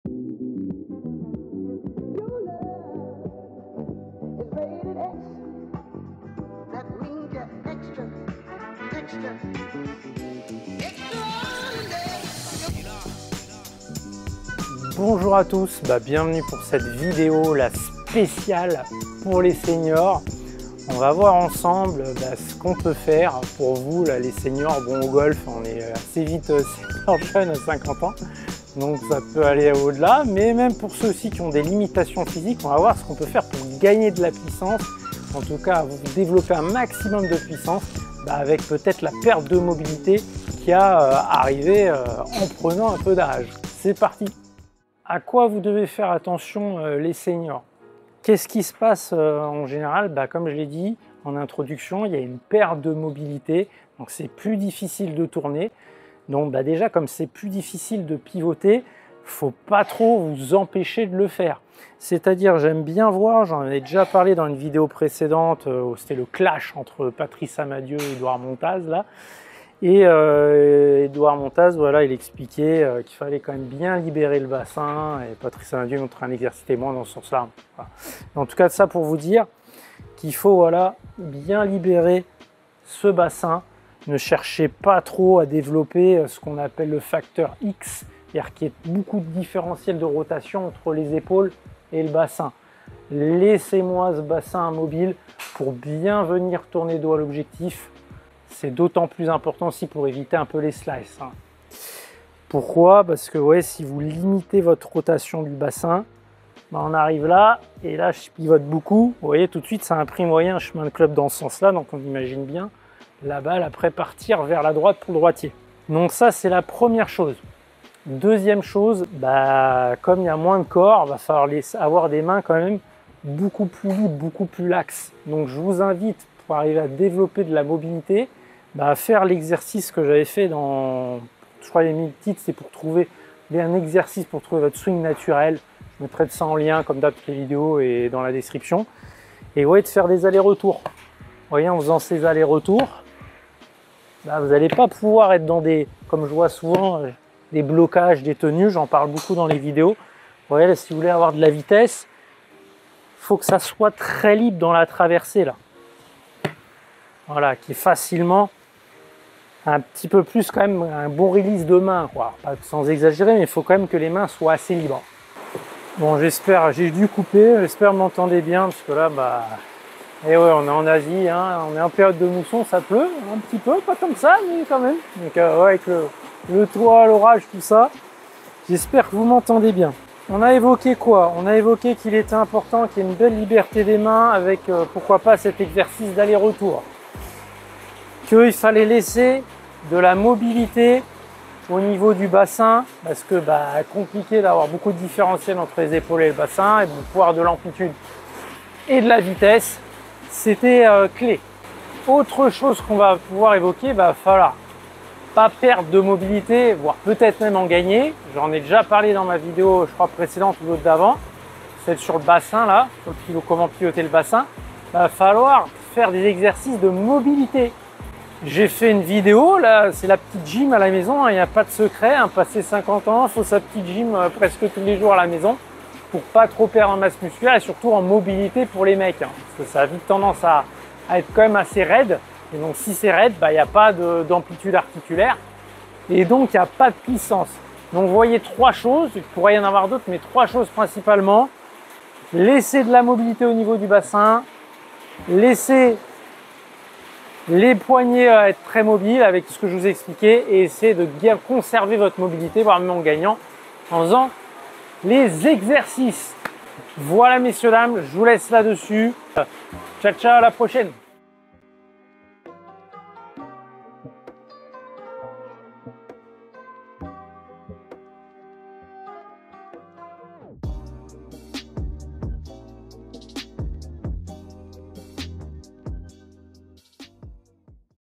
Comment les Seniors (+ de 50 ans) peuvent gagner en Puissance ?
Les seniors n’ont souvent pas les mêmes capacités physiques qu’un homme de 30 ans. Il sera certainement nécessaire de déverrouiller le bassin pour favoriser la rotation, améliorer le relâchement dans la traversée de la balle pour ne pas perdre en vitesse et faire des exercices pour conserver, voire développer, la mobilité. Je vous explique tout ça dans cette vidéo.

_______VIDEOS EVOQUEES________